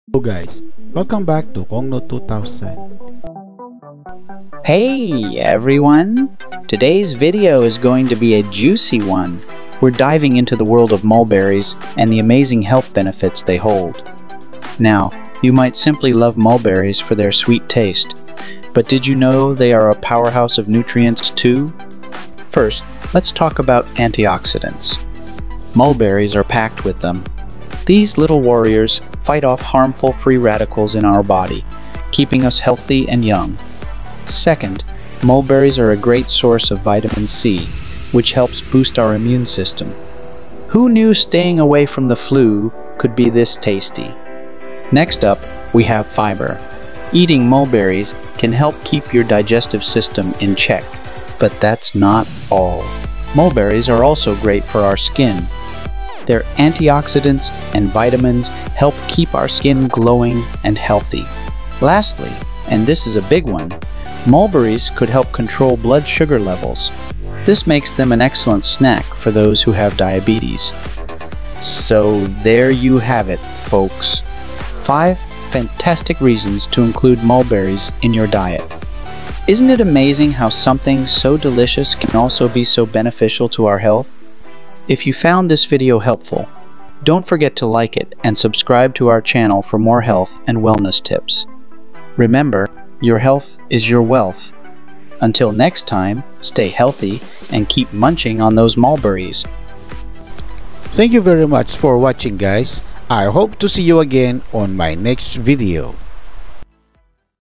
TOP 5 HEALTH BENEFITS OF EATING MULBERRIES
this video is about the health benefits of eating fruits edited manually using filmora and automatically using videozen.ai. and veed.io